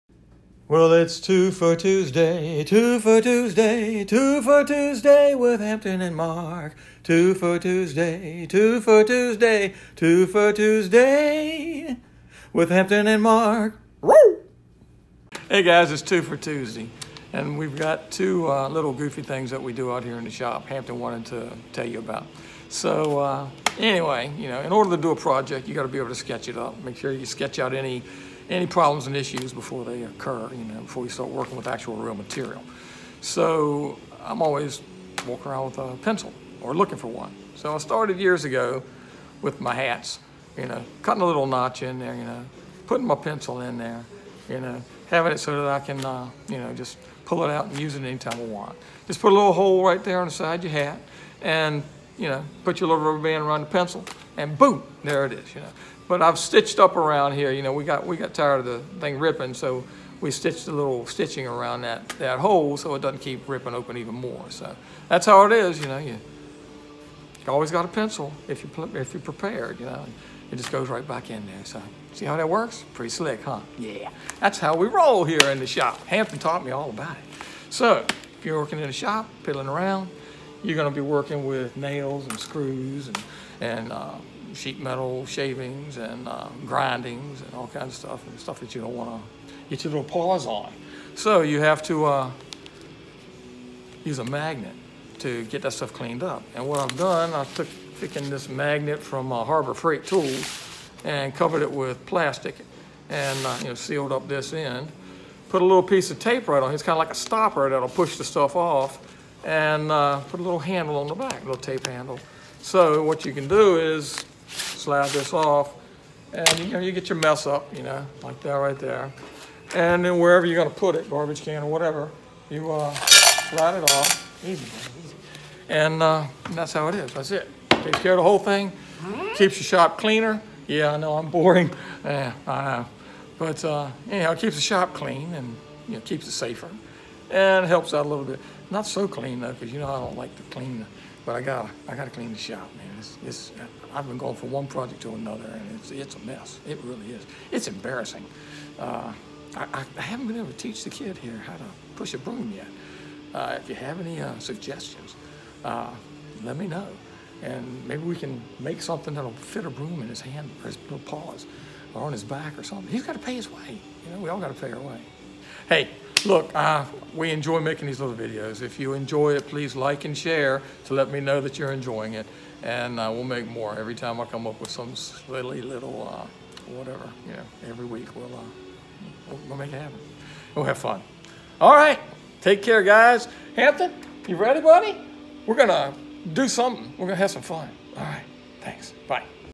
SHOP TIPS - Two for Tuesday with Hampton & Mark - 020
Come with Hampton & Mark as they give you Shop Tips to make your fun time more enjoyable...